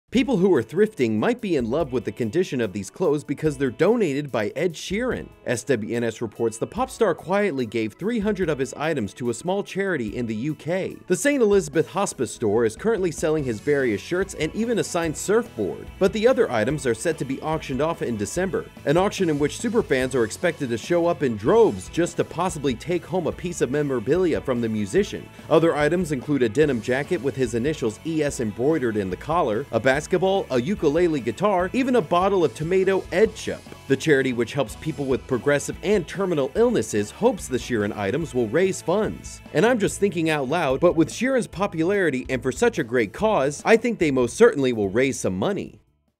Ed Sheeran Donates 300 Items To Charity Store Which Fans Will Flock To In Droves!
People who’re thrifting might be in love with the condition of these clothes because they’re donated by Ed Sheeran! Buzz60’s Mercer Morrison has the story.